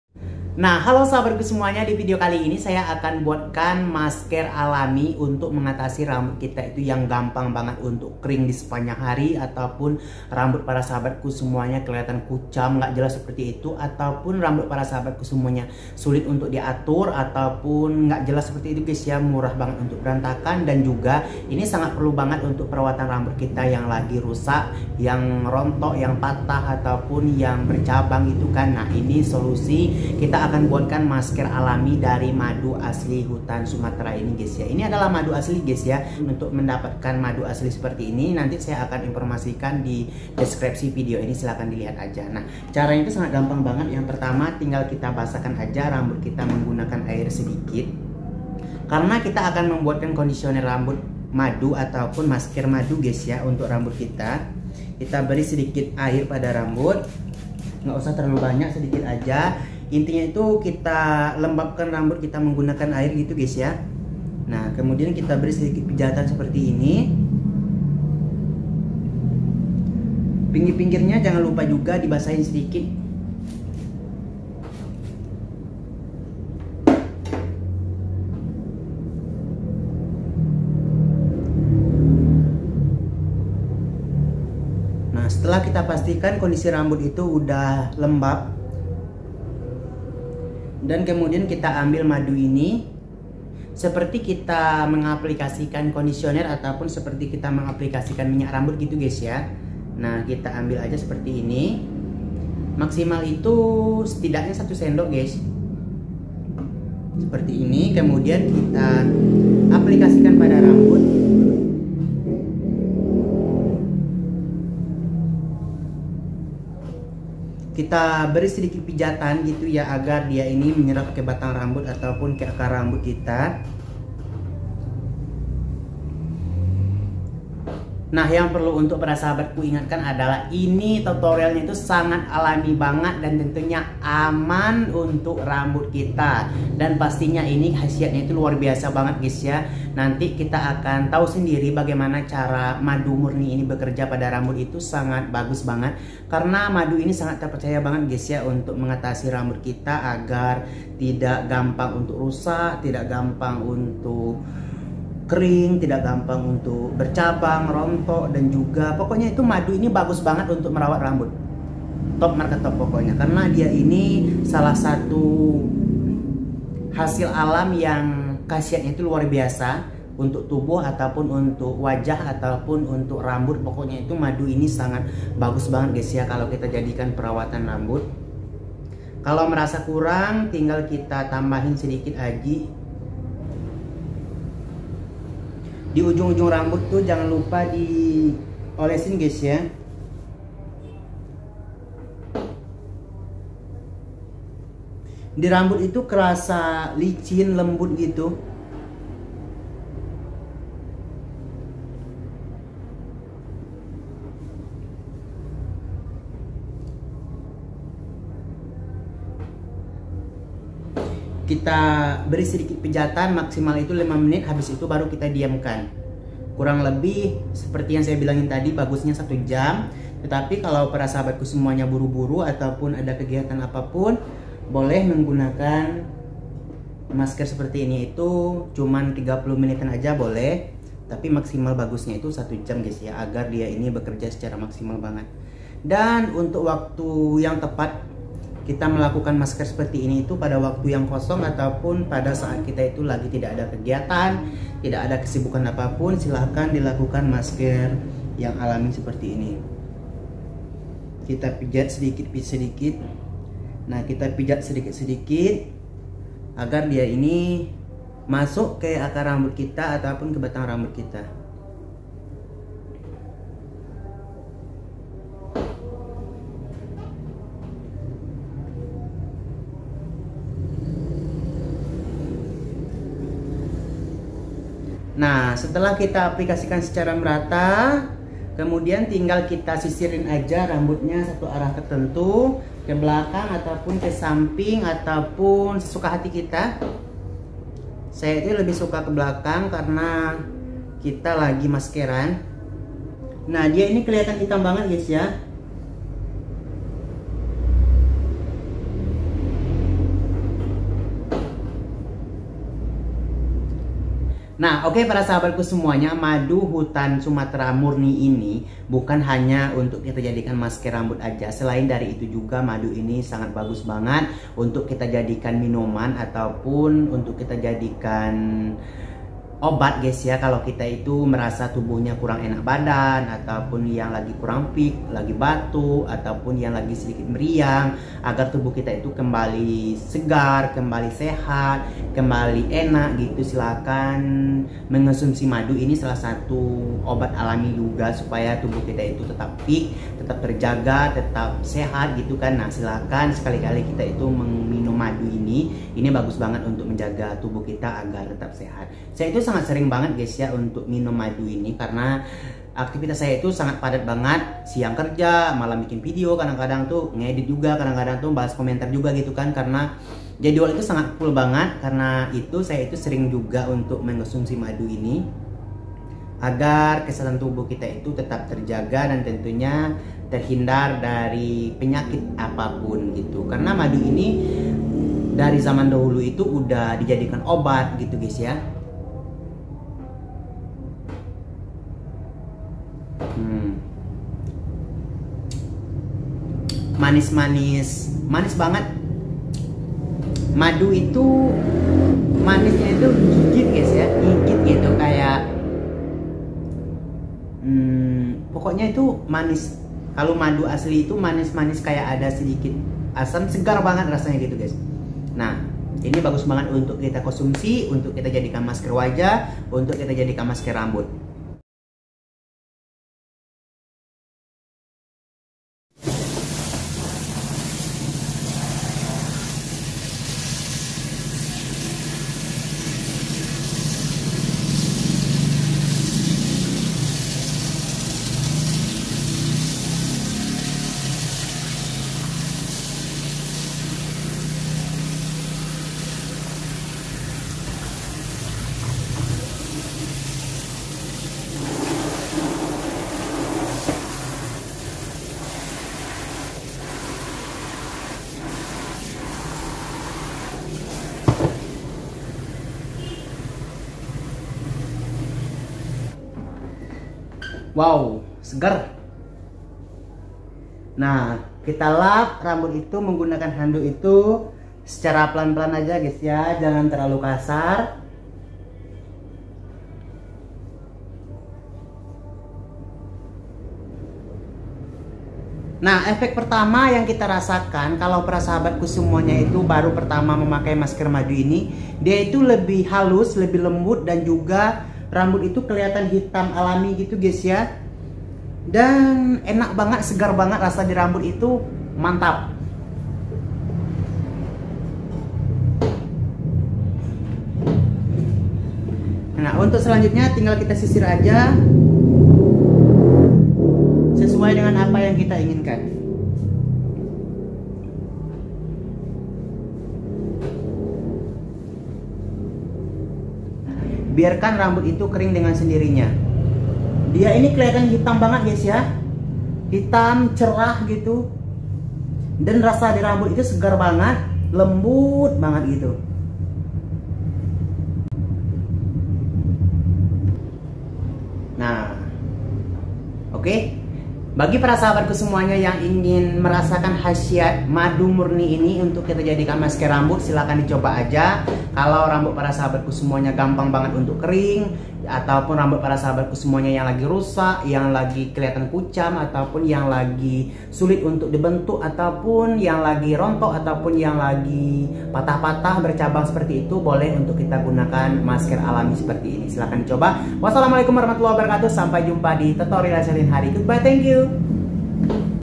cara mengatasi rambut kering dan rusak pria | secara alami
madu murni ini sangat bagus Banget untuk mengatasi rambut kering, rusak kusut dan sulit untuk dibentuk. dan masih banyak juga yg lainnya.
madu murni seperti ini bukan hanya bagus untuk rambut.
tapi juga bagus untuk dikonsumsi untuk kita jadikan minuman untuk menjaga kesehatan tubuh.
selain itu juga bisa untuk kita jadikan masker wajah.
untuk info pembelian.